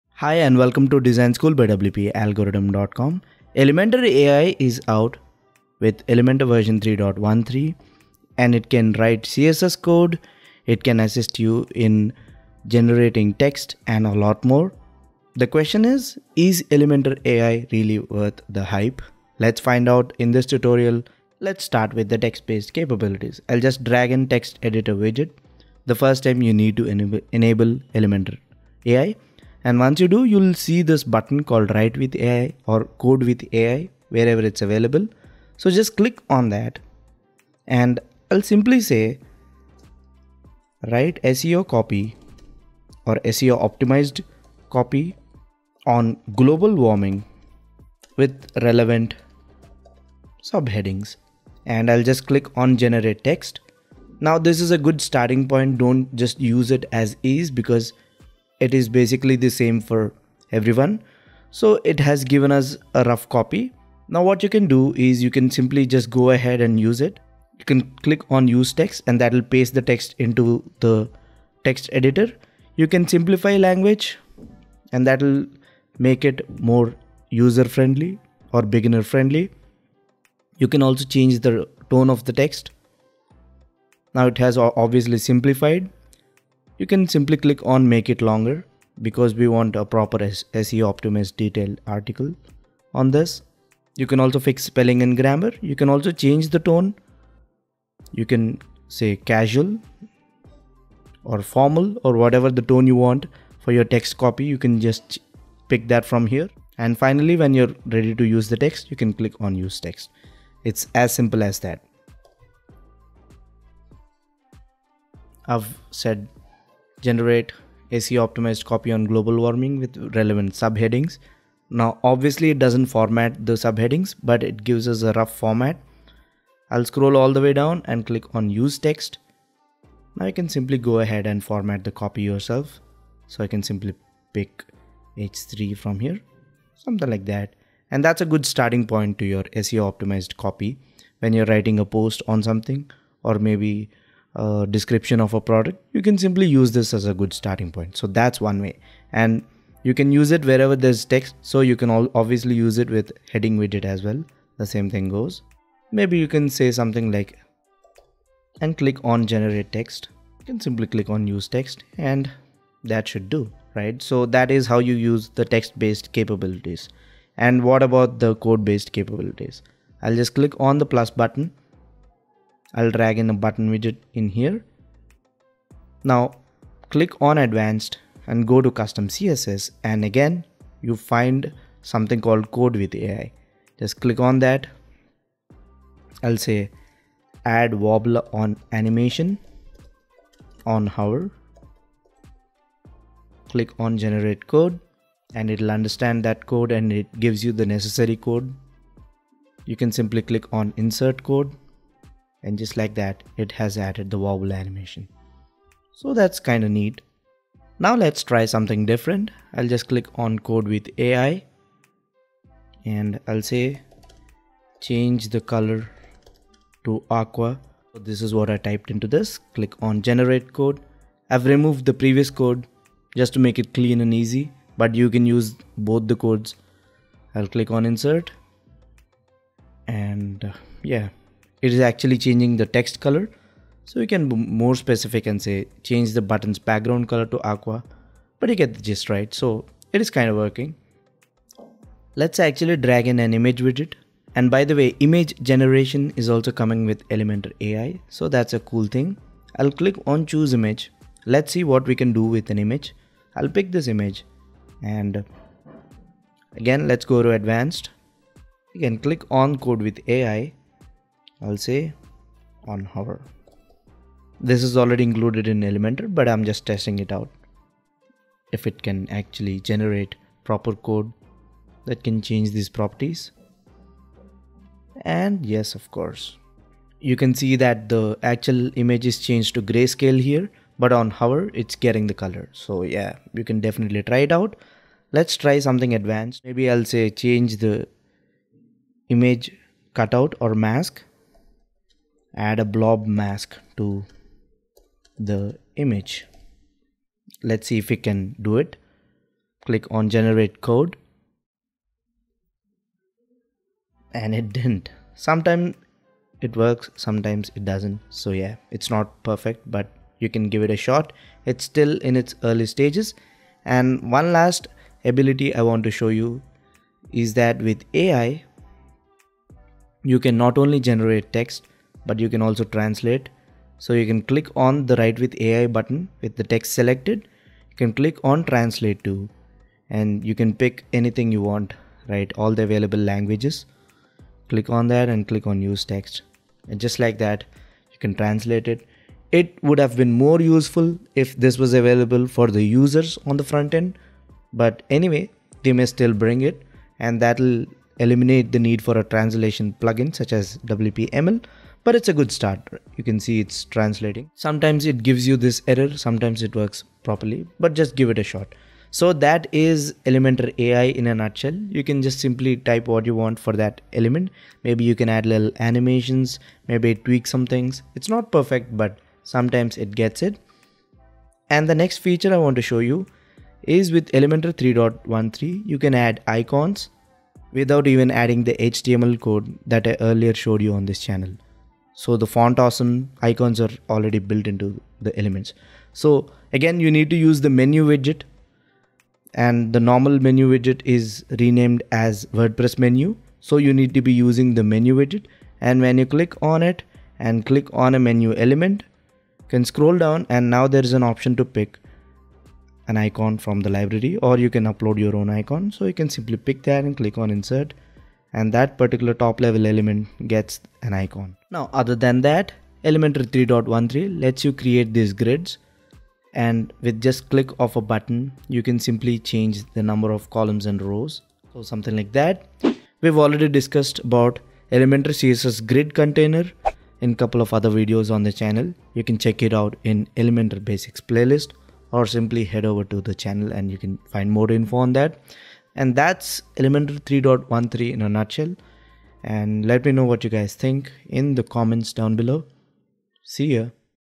IS Elementor AI USEFUL or a Marketing Gimmick?! (New Elementor 3.13 Features Explained)
Learn  about Elementor AI and other features in this Elementor 3.13 tutorial (PRO VERSION TOO).

Master CSS Basics for Elementor with my Udemy Course